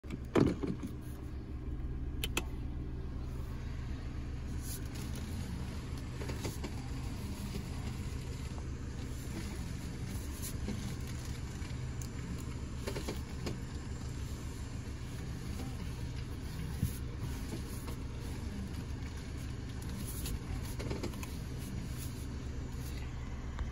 Mazda3 : Creaking noise when turning the wheel
Mazda 3 2021 8K km.  Creaking noise when turning the wheel when stationary and at really low speed. It only happens when in Drive.